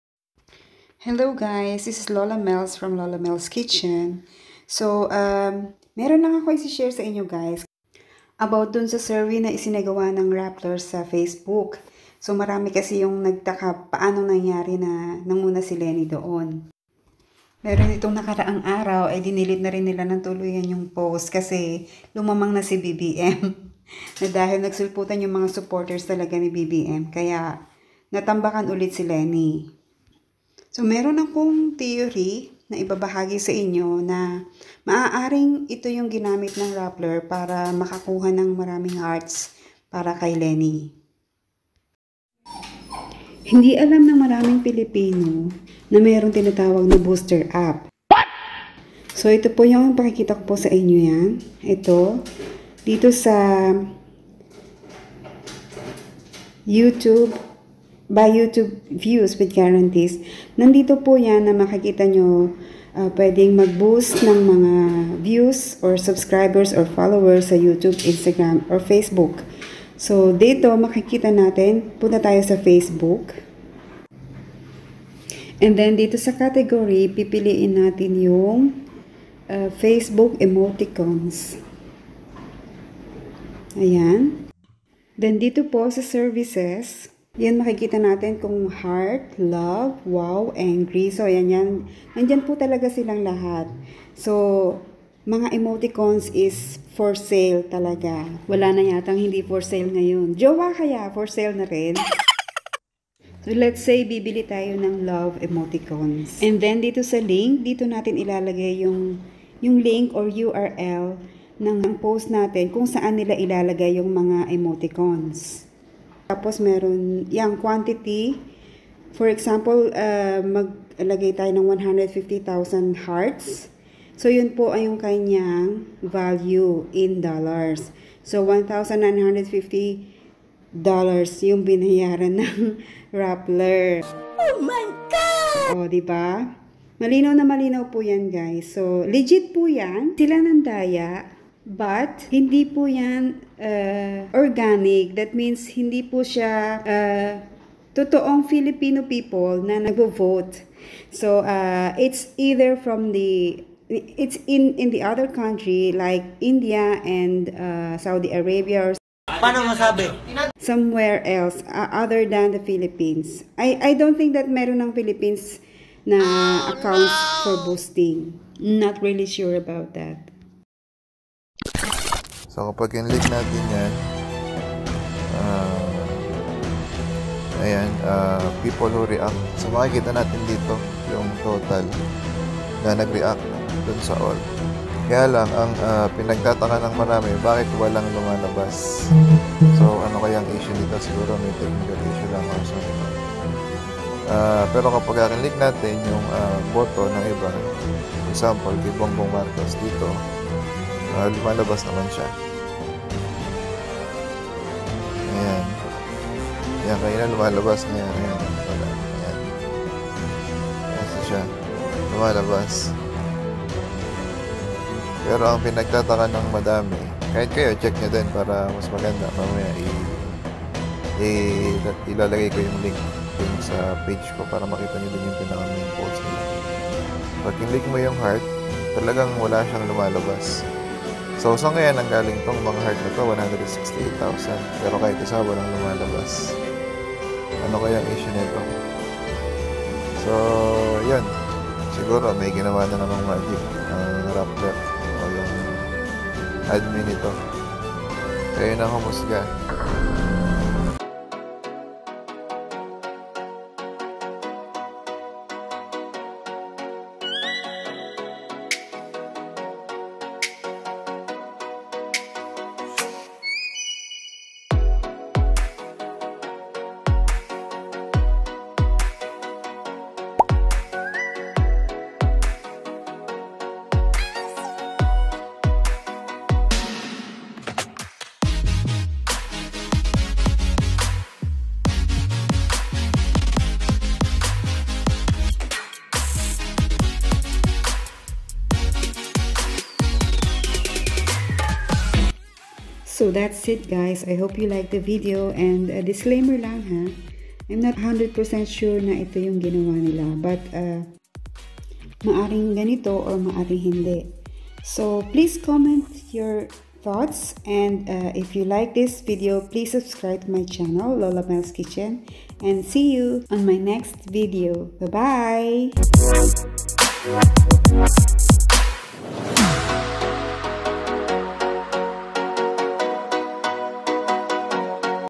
ALAMIN:POSIBLENG PANDARAYA NA GINAWA NG RAPPLER SA PRESIDENTIAL SURVEY/ MAGKANO KAYA?
Paano namanipula ng rappler ang hearts emojis sa facebook? Alamin ang possibleng taktika ng rappler ukop dito.